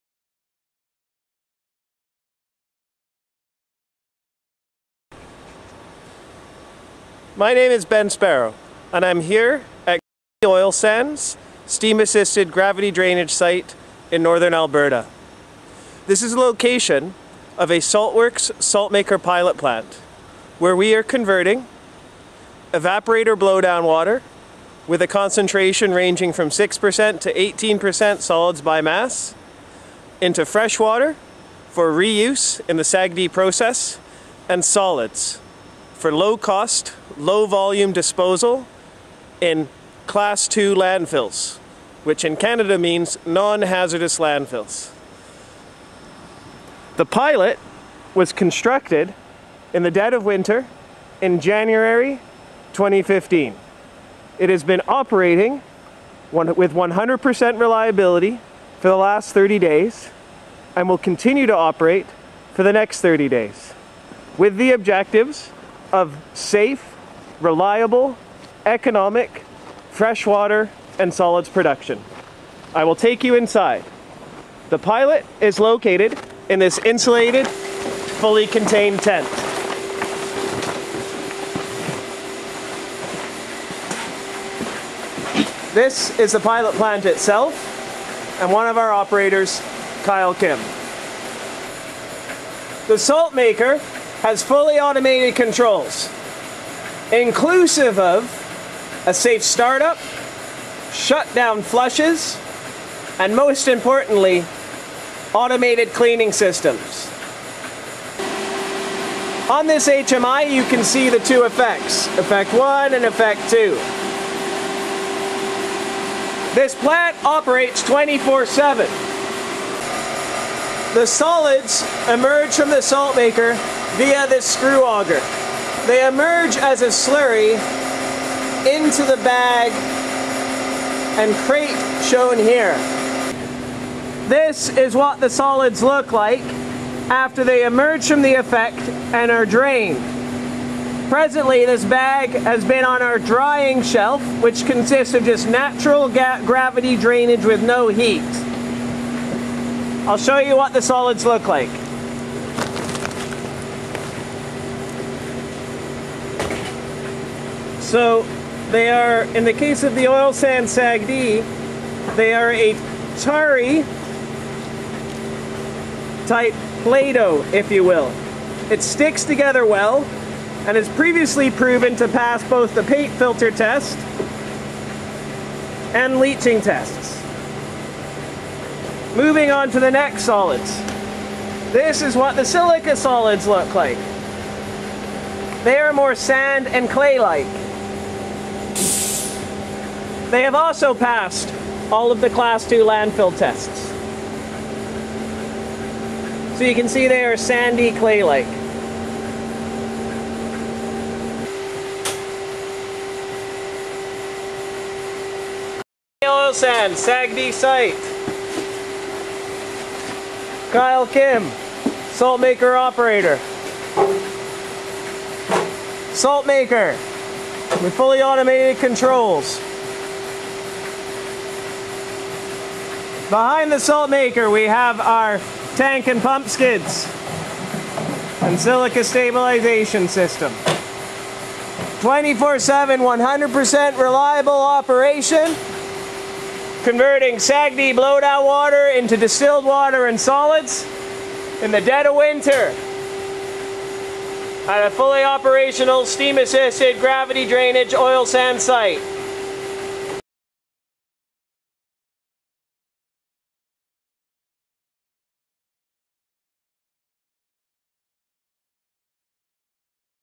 Saltworks' SaltMaker Evaporator Crystallizer Pilot Treating SAGD Oil Sands Evaporator Blowdown
Saltworks Technologies Inc.

Saltworks' SaltMaker Evaporator Crystallizer pilot plant operating at a Steam Assisted Gravity Drainage (SAGD) oil sands site on evaporator blowdown wastewater. This pilot SaltMaker Evaporator Crystallizer will be treating SAGD evaporator blowdown wastewater to produce freshwater for reuse and solids for true zero liquid discharge (ZLD), providing brine management and greatly reducing the operator's trucking and disposal costs.

The SaltMaker is a low temperature evaporator crystallizer wastewater treatment plant. Input low grade heat and your wastewater or brine. Produce two outputs: freshwater and all contaminants reduced to solids for true zero liquid discharge.

Applications:
Reduce off-site disposal costs
Improve crystallization reliability
True zero liquid discharge
Landfill leachate
RO brine treatment, solids production
Oil and gas produced water
Shale gas fracking water
Evaporator brine and blowdown